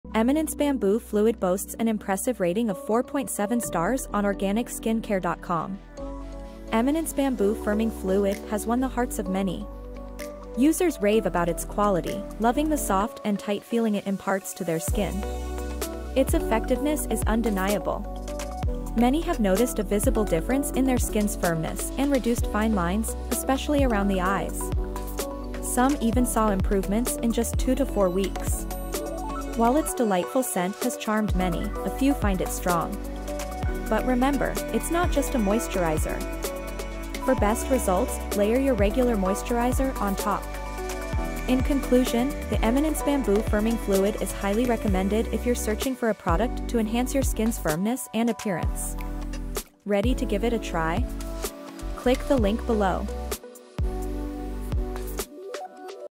Eminence Organics Bamboo Firming Fluid Review [ Watch This Vvdeo Before You Buy It ]
Eminence Bamboo Firming Fluid fills in fine lines and wrinkles with the use of Natural Retinol Alternative and Swiss Green Apple Plant Cells. Formulated with Bamboo, Eminence Firming Fluid utilizes soluble and insoluble fiber, antioxidants, proteins, vitamins, and minerals to provide skin with a more radiant, youthful appeal.